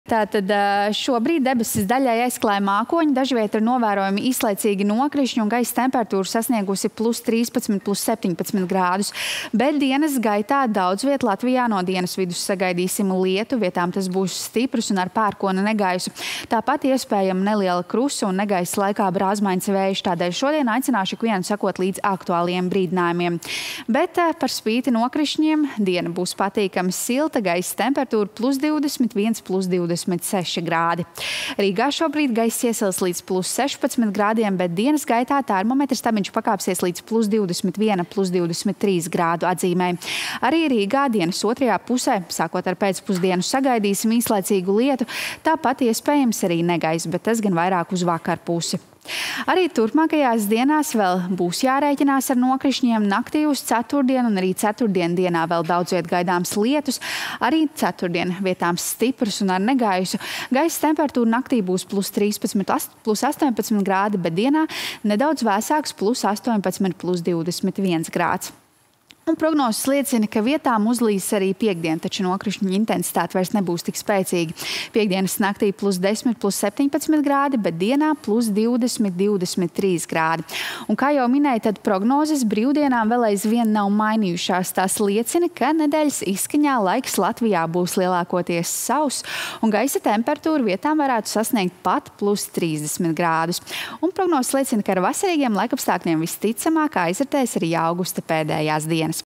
21. augusta laika ziņas
Trešdien Latvijā būs visai mākoņains laiks un daudzviet gaidāms īslaicīgs lietus. Vietām līs stipri un dārdēs pērkons.

Gaisa temperatūra paaugstināsies līdz +20..+25 grādiem.